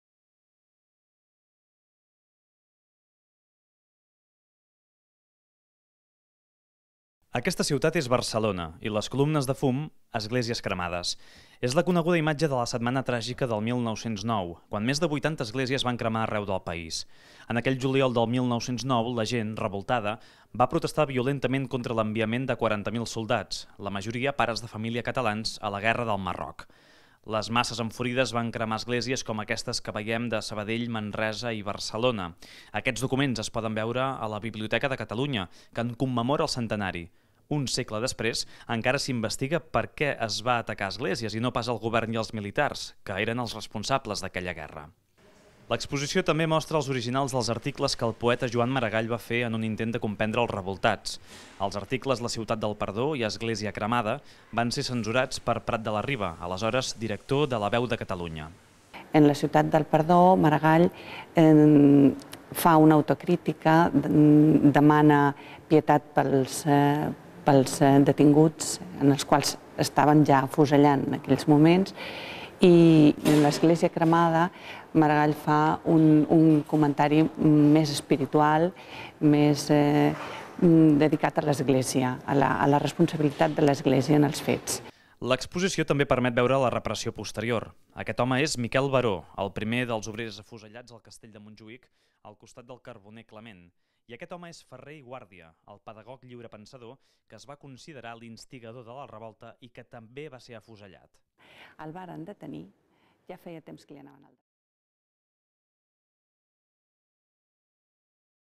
Les imatges de la Setmana Tràgica
El contingut complet d'aquest vídeo el podeu veure a VilaWeb TV

La Biblioteca de Catalunya exposa aquests dies els materials del seu fons referents a la Setmana Tràgica: els articles originals de Joan Maragall fent autocrítica, imatges d'esglésies cremades o la repercussió a la premsa internacional.